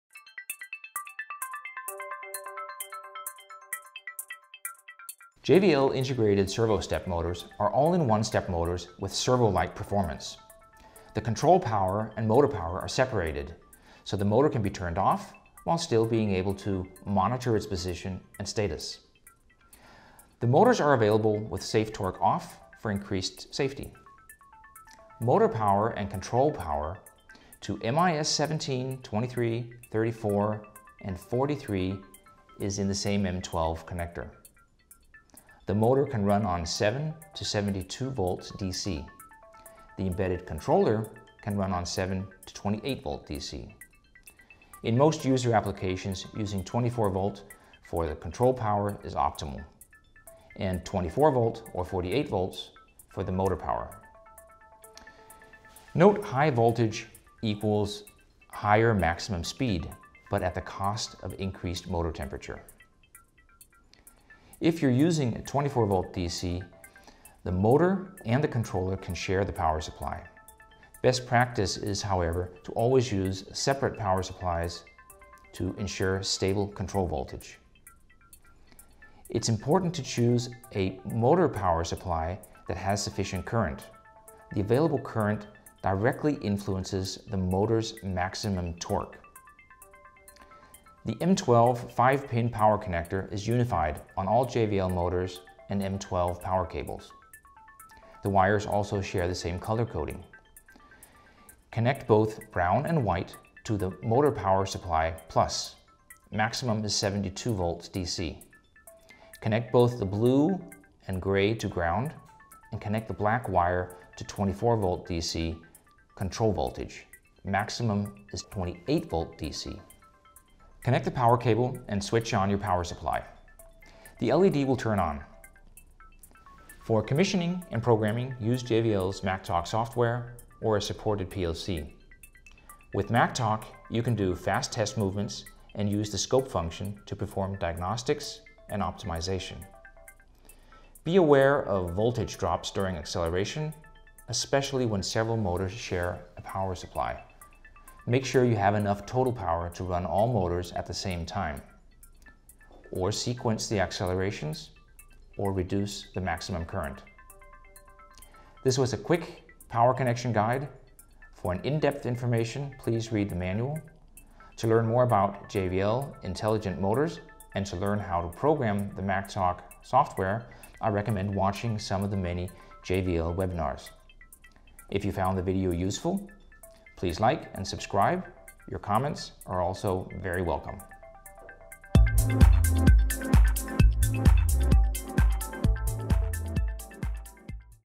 JVL Integrated Stepper Motor - Connection guide (MIS17 - 43)
Watch Kenneth guide you through how to connect your JVL MIS17 to 43 Integrated Stepper Motor.